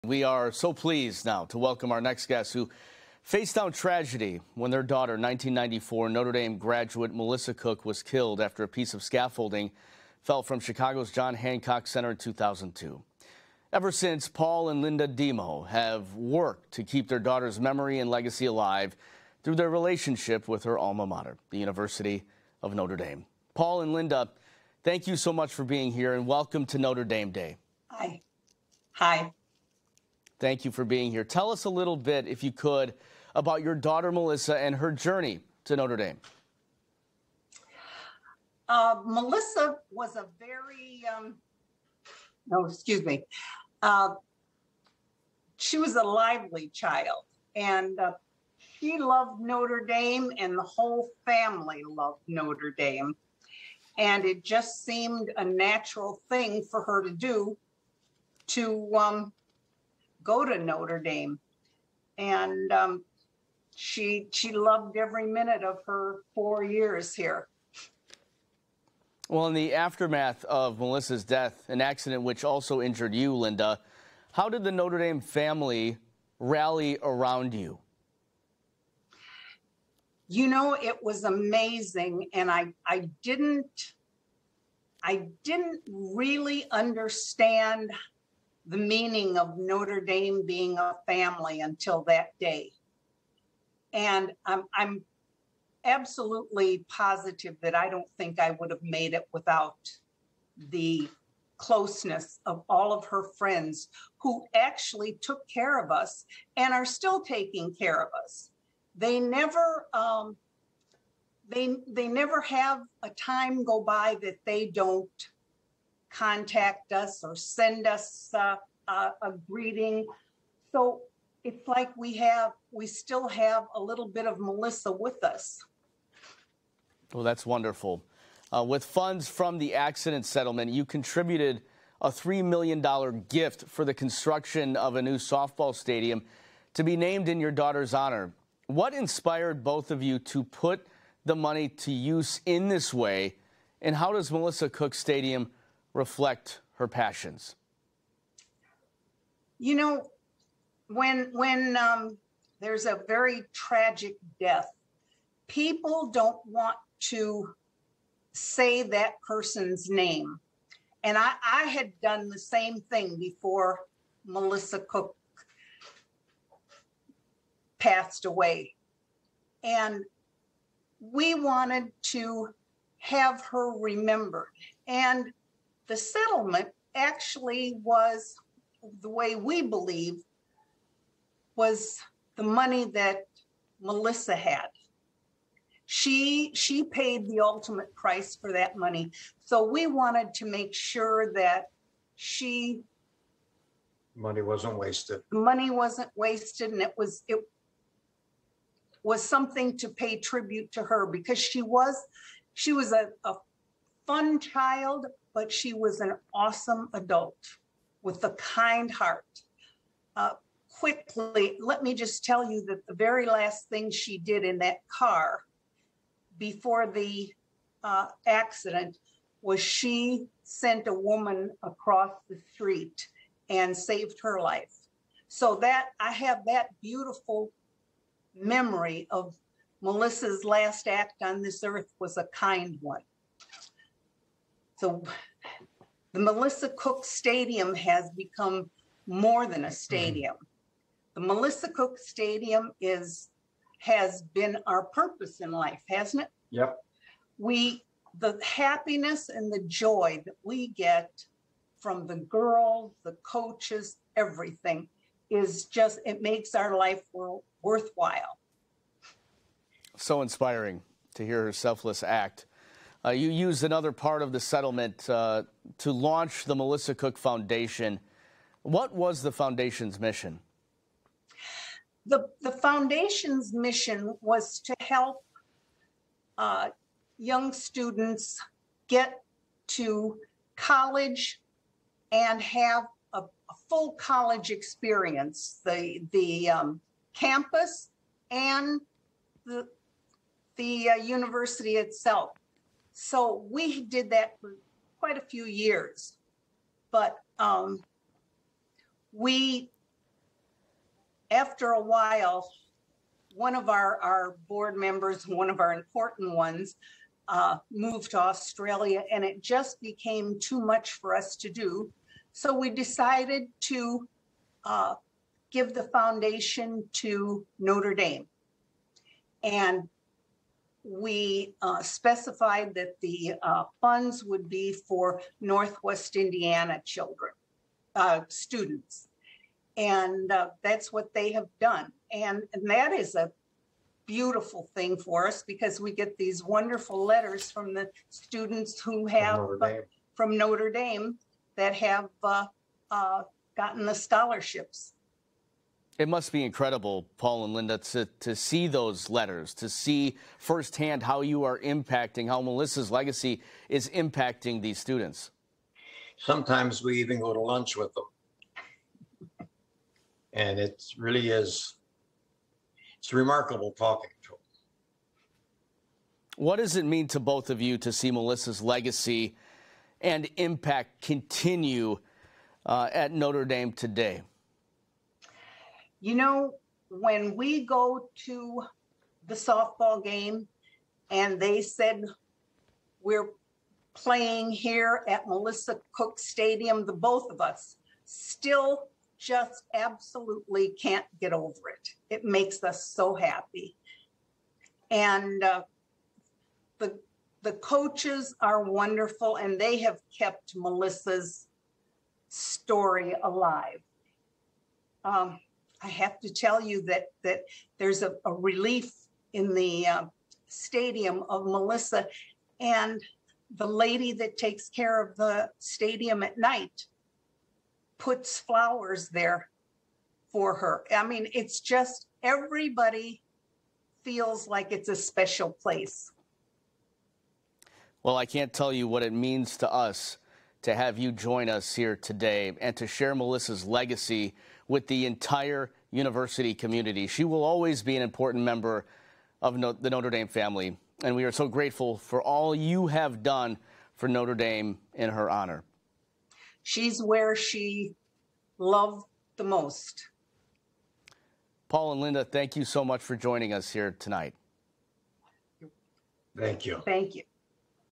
Paul & Linda Demo — Notre Dame Day 2021
Notre Dame parent’s Paul and Linda Demo share how the Notre Dame Family has embraced them since the tragic loss of their daughter, Melissa Cook ’94, and how they have made sure her memory lives on here at Notre Dame.